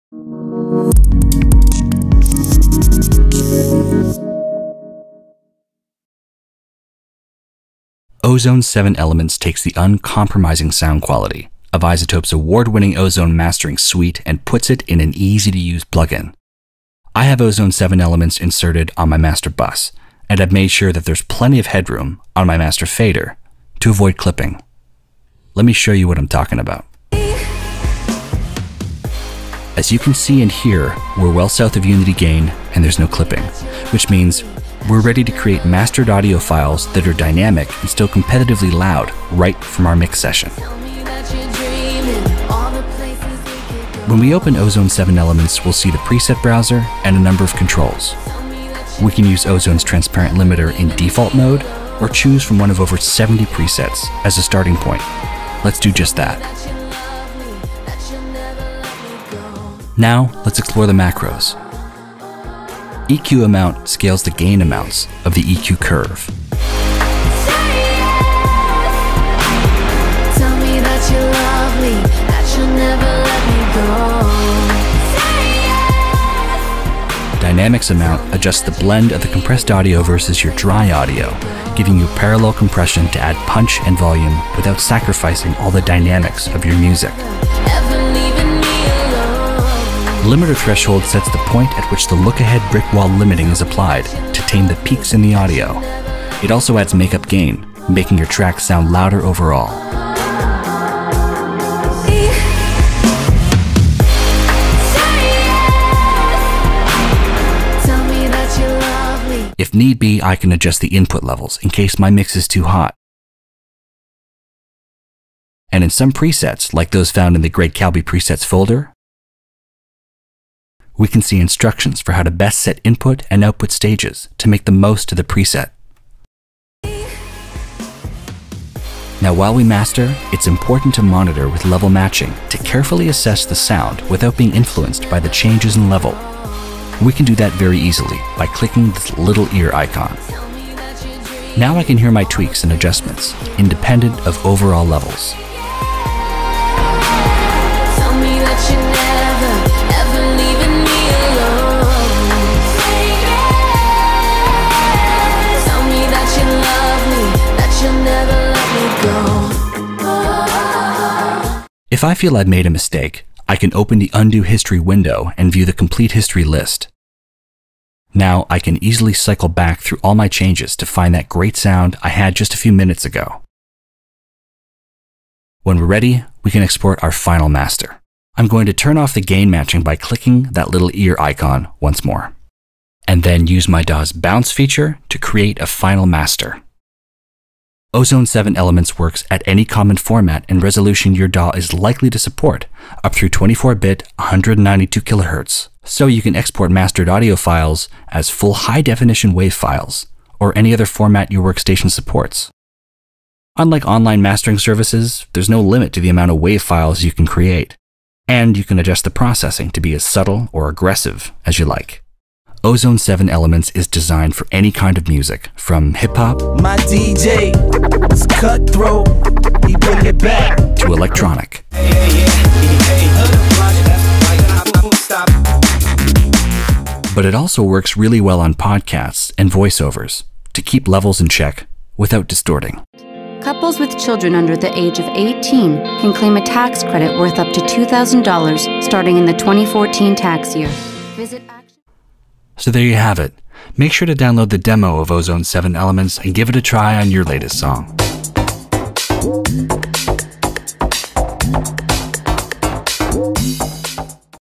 iZotope Ozone 7 Elements - Features Overview & User Guide
A short overview of the new Ozone 7 Elements plugin and feature set, and some examples of it in use.

Effortlessly get your mixes radio and streaming-ready with Ozone 7 Elements. With industry standard processing from Ozone 7, this new mastering plug-in enables you to achieve a full, rich, and loud final mix with professionally-crafted presets and simple macro controls to shape your sound.

Masterfully Simple

With professionally designed, goal-oriented presets and simple, high-impact macro controls, it’s never been easier to achieve a final mix with the sonic impact and energy of songs from your favourite albums.

Presets

Start by selecting a preset. Under the hood, each mastering preset drives a series of Ozone processors designed to bring your tracks up to radio-ready quality. Choose from Genre-Specific, All-Purpose, or Artist Signature presets to zero in on your desired aesthetic.

Amount Sliders

Tune your track to your taste with the EQ and Dynamics Amount sliders to shape the polished sound you’re after.

Maximiser Threshold

Bring your tracks up to a competitive level with Ozone’s legendary Maximiser. Make your tracks louder without clipping or sacrificing clarity or punch, and ensure you have sufficient headroom for streaming and MP3/AAC encoding.